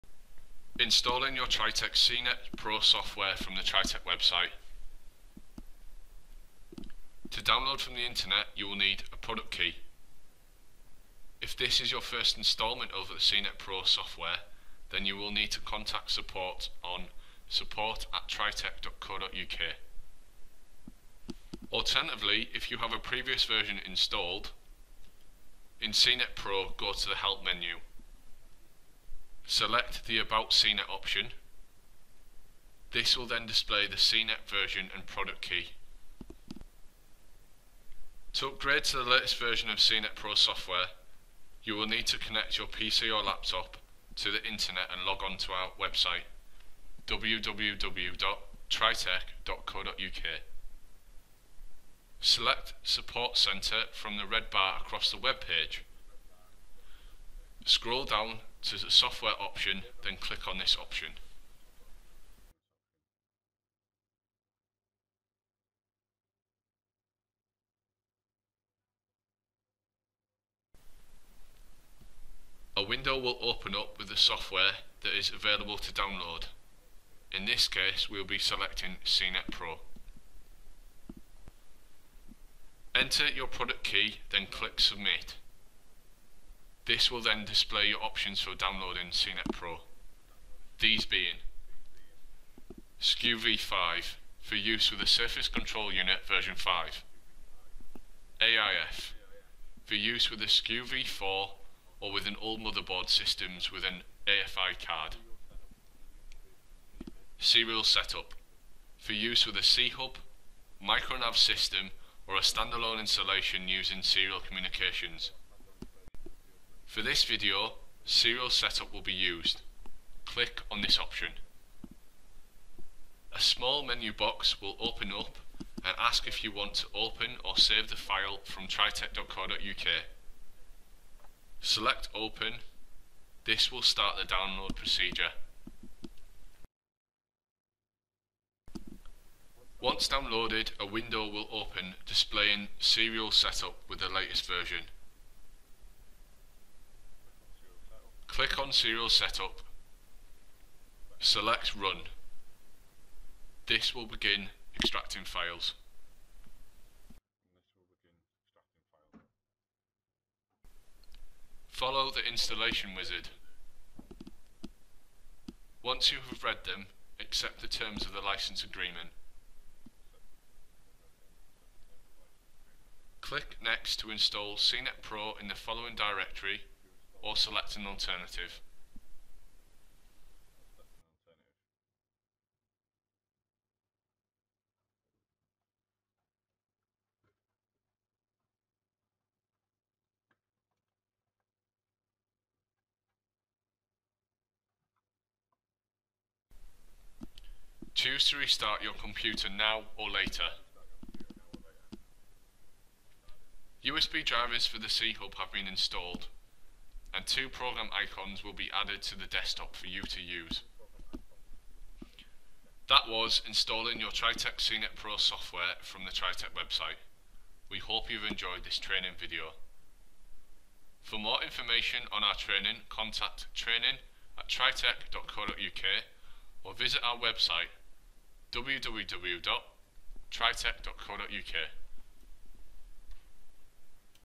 Installing Seanet Pro Software from the Tritech Website
An instructional video showing how to download and install the latest version of Seanet Pro Software.